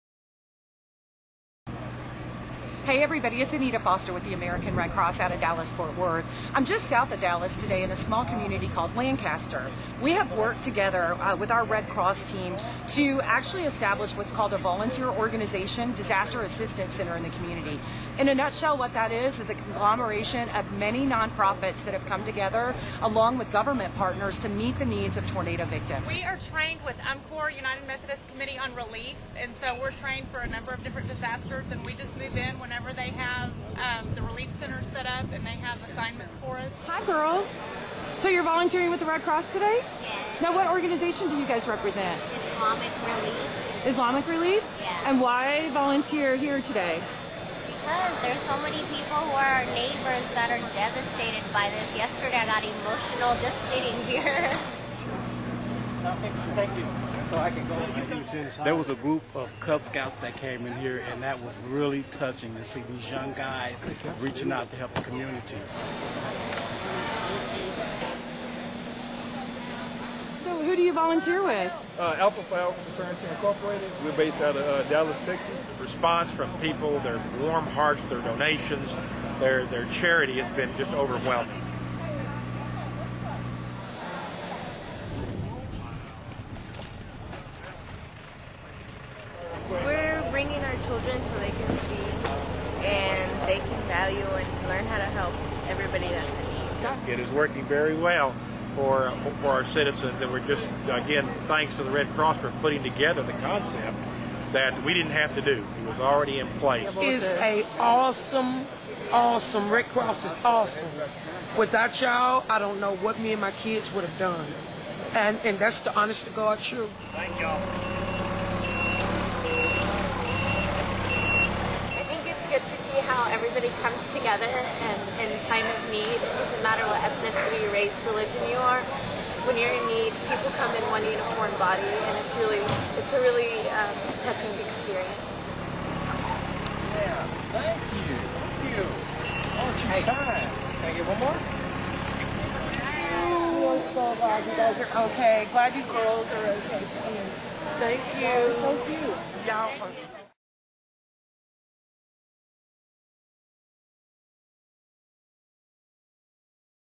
American Red Cross Brings Partners Together for Tornado Relief.wmv
On the afternoon of April 3, 2012, 16 tornadoes went on a rampage across the Dallas-Fort Worth Metroplex. In the immediate aftermath of the storms, the American Red Cross opened shelters, distributed thousands of meals, snacks and clean-up supplies. Additionally, Red Cross teams worked with city officials to open "one-stop shops" for tornado victimcs in Lancaster and Arlington, Texas.

At the Disaster Assistance Centers, families are able to meet with numerous organizations, all with the aim of helping them get on the road to recovery. This video highlights just a few of the groups helping Texans recover.

Video shot by Anita Foster and Rusty Surette, American Red Cross